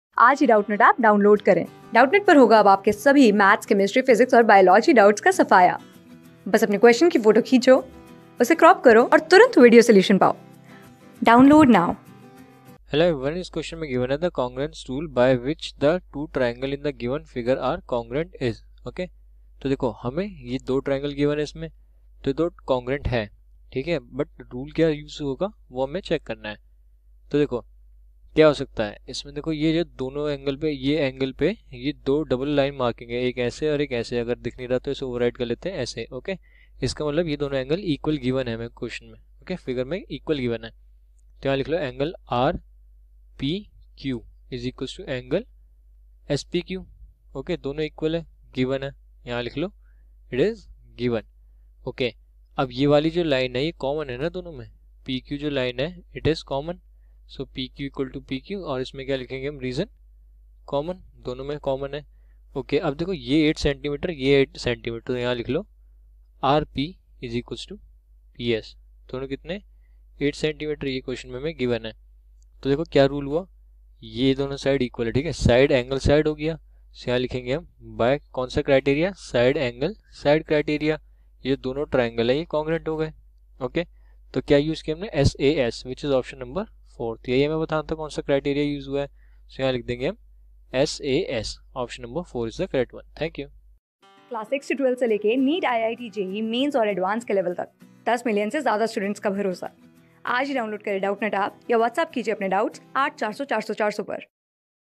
The congruence rule, by which the two triangles in the given figure are congruentis ____________...
Class: 9
Subject: MATHS
Chapter: TRIANGLES
Board:IIT JEE

👉 Have Any Query? Ask Us.